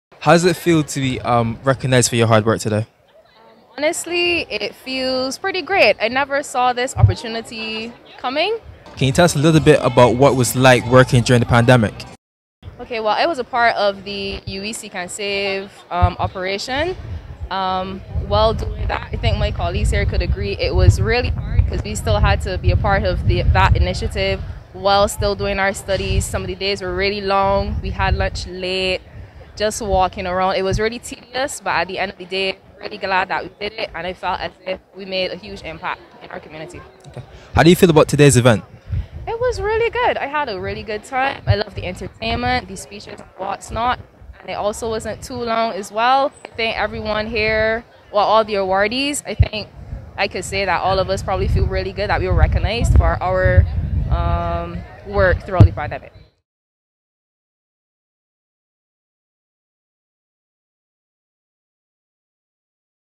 Nation Update: Humanitarian Awards
Shanika Roach was part of the University of the West Indies’ Operation Seek and Save during the COVID-19 pandemic.
She was among many honoured during the Humanitarian Awards Ceremony, National Heroes Day, at Kensington Oval, April 28, 2022. (Video by Reco Moore)